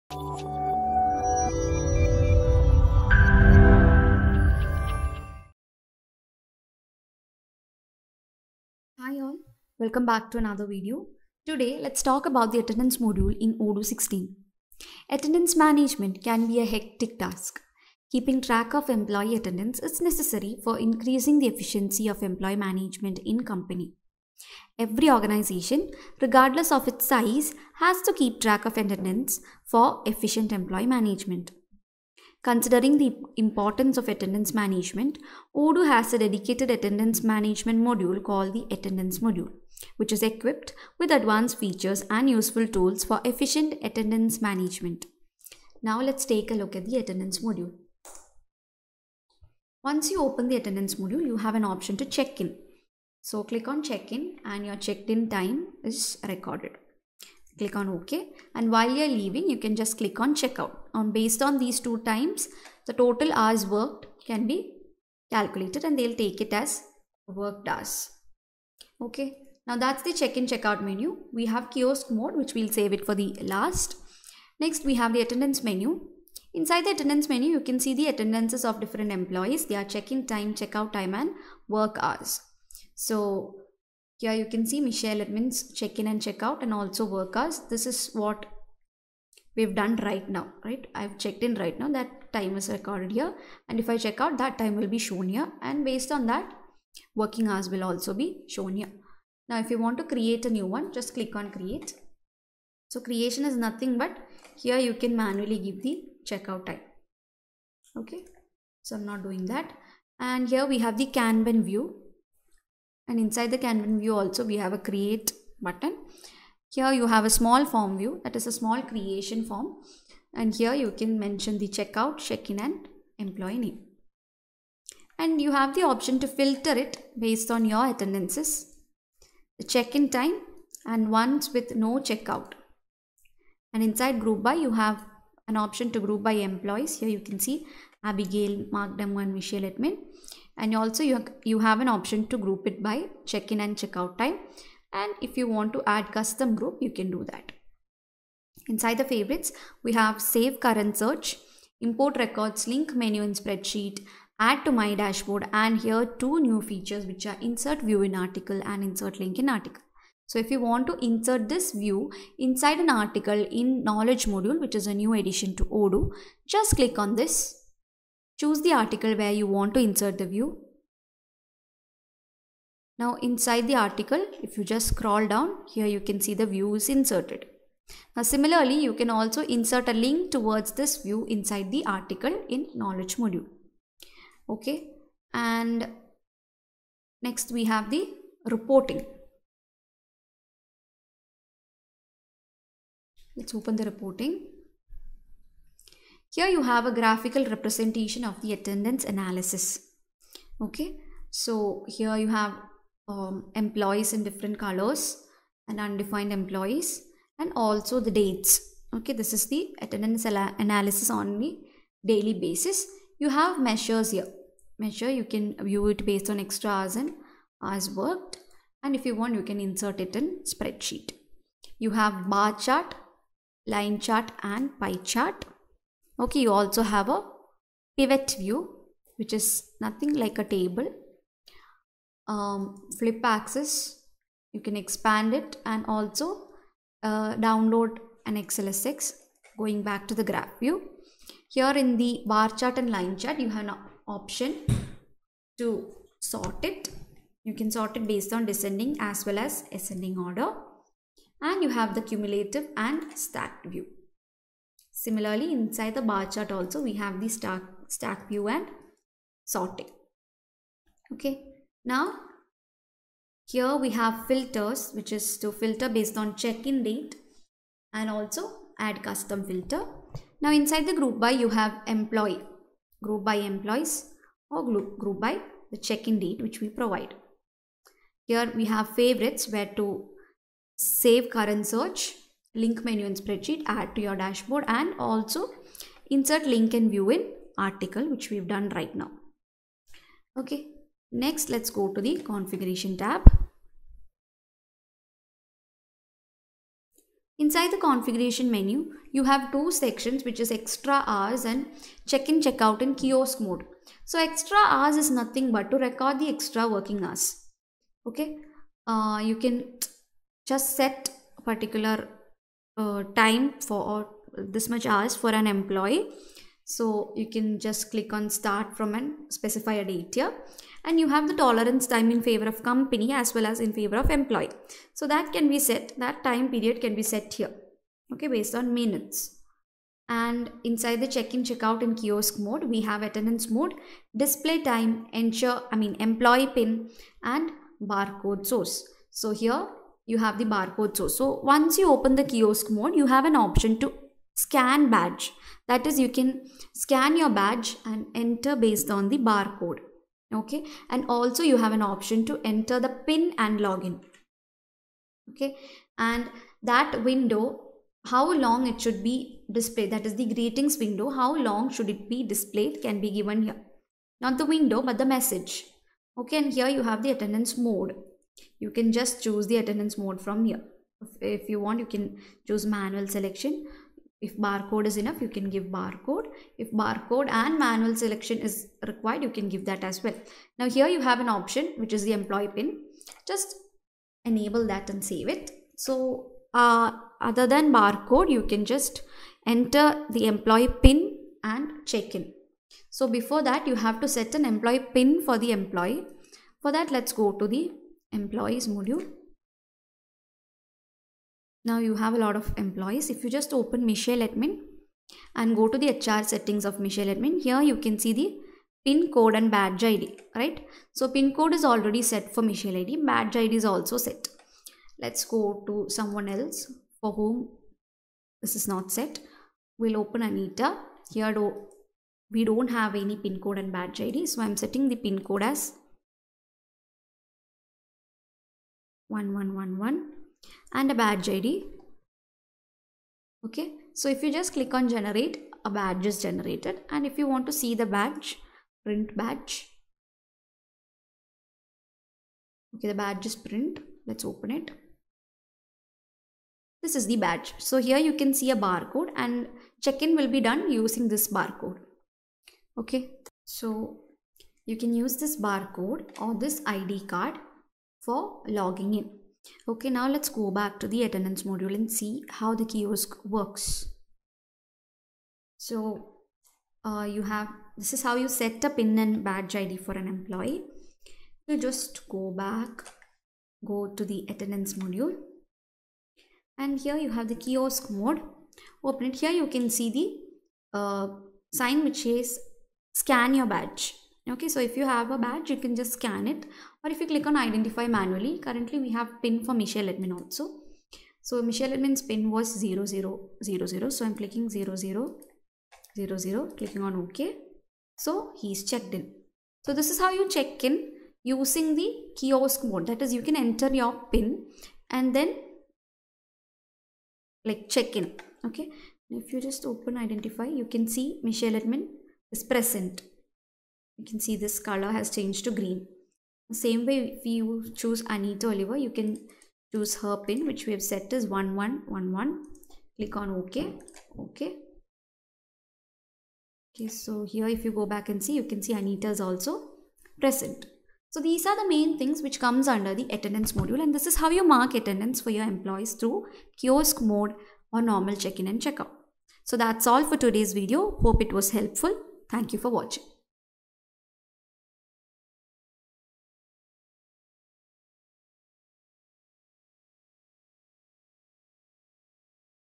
Odoo 16 Attendance Module | Odoo 16 Enterprise Edition
This video explains about the Attendance module in Odoo16. Keeping track of Employee Attendance is necessary for increasing the efficiency of employee management in a company.

Video Chapters
00:00 Introduction
00:48 Check-in/ Check Out
01:38 Create Attendance
01:49 Views, Filter, Groupby, Favorites
03:04 Reporting
04:50 Configuration
06:32 How to Set Pin and Badge to an Employee
07:50 Kiosk Mode

Every organization regardless of its size has to keep track of attendance for efficient employee management.